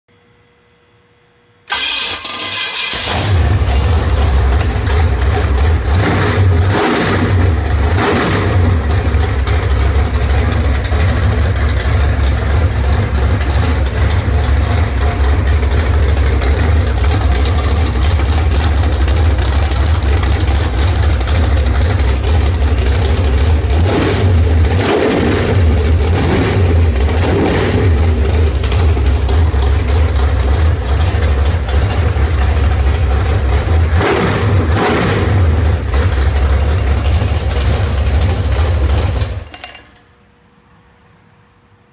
Sandoval Performance 488 tripower Pontiac engine
Initial start up. Engine utilizes our Stage 1 CNC ported Edelbrock aluminum cylinder heads, a slightly customized version of our Stage 4 "Pro-touring" hydraulic roller camshaft, 4.5" stroke Eagle forged rotating assembly and 4 bolt mains.